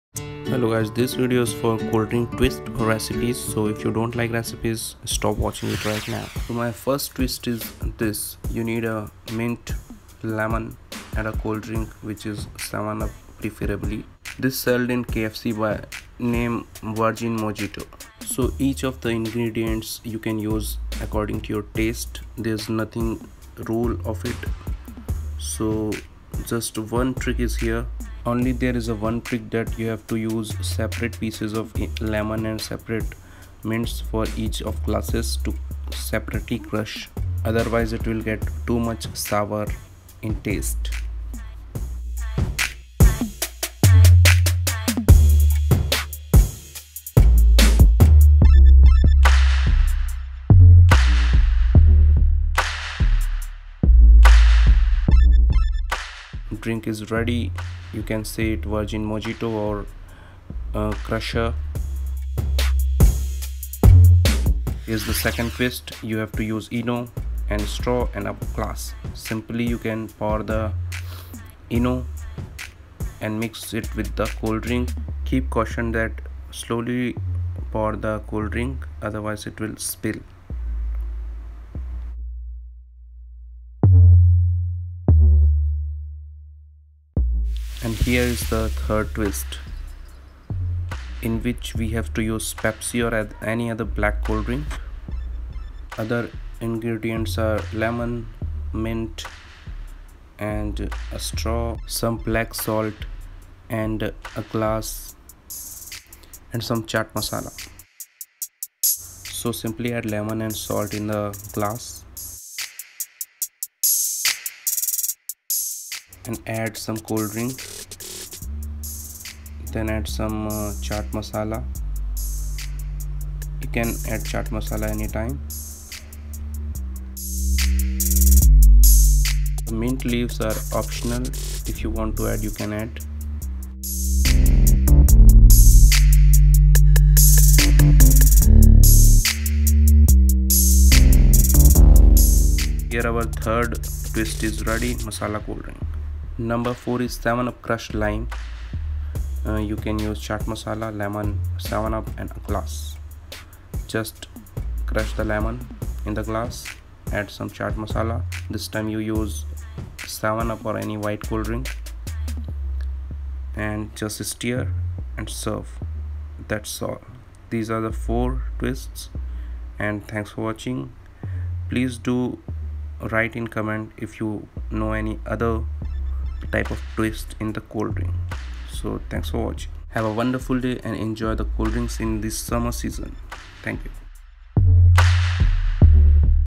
VIRGIN MOJITO ||MASALA COLDDRINK RECIPE
bored of colddrink ,here some recipe to change the taste
you just need colddrink, lemon, mint ,chat masala,black salt,ENO